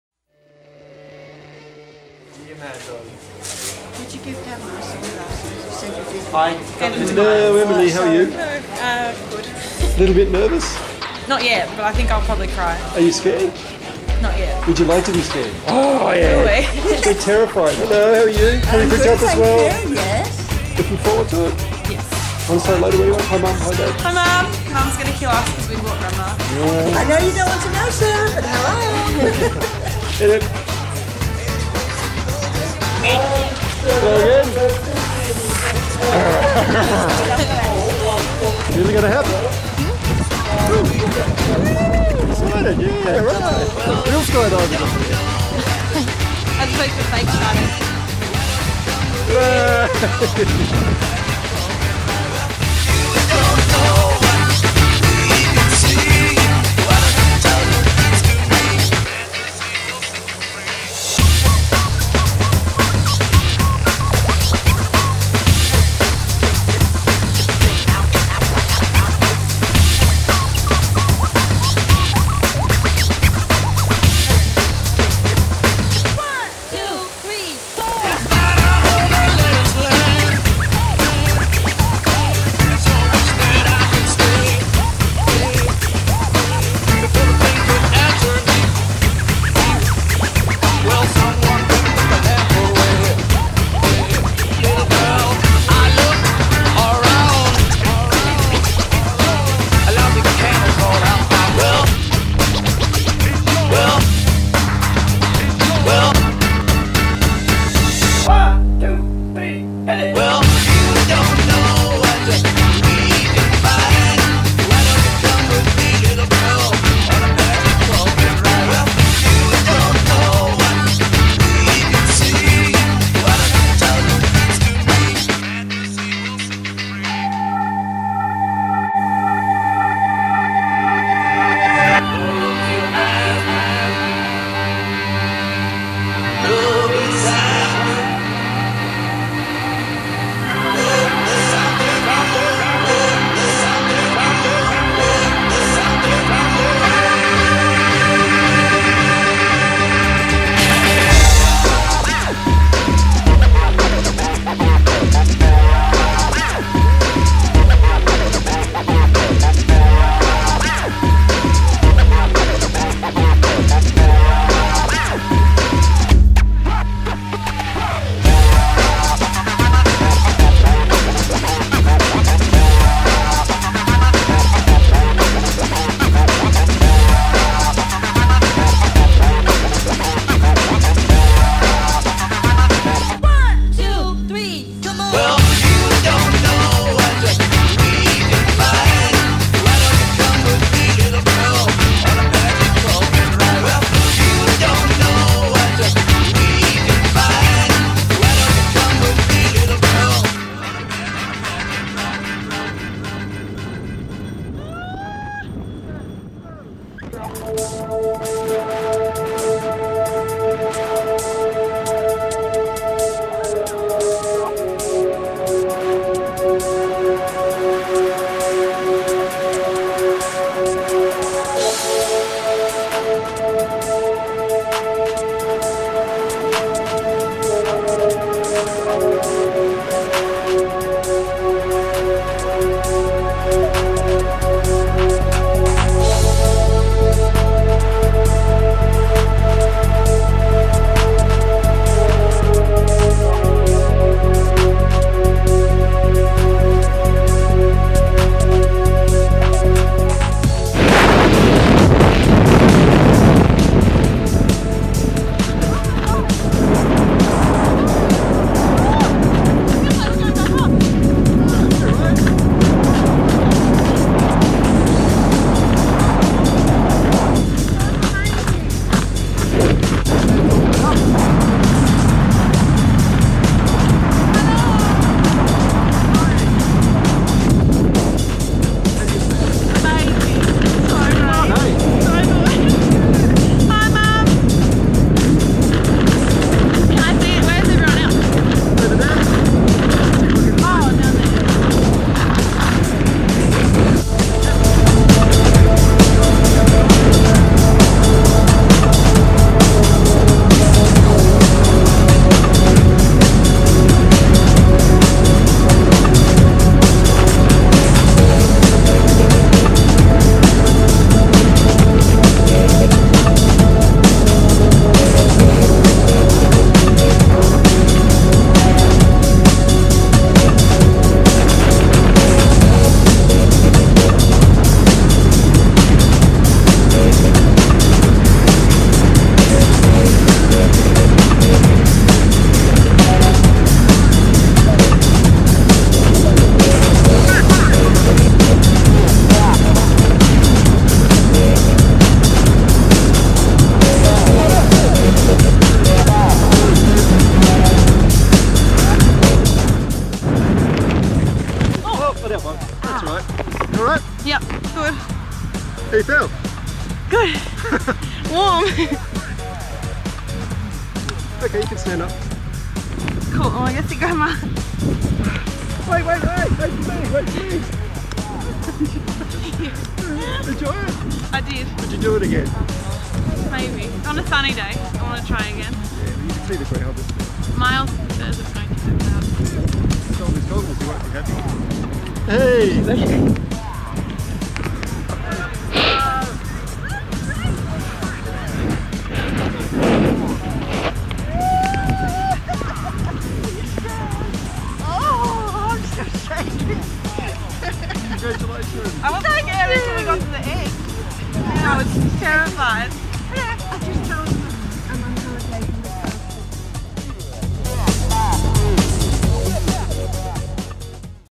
Emily's Fall From The Skyvan.
Emily takes her first tandem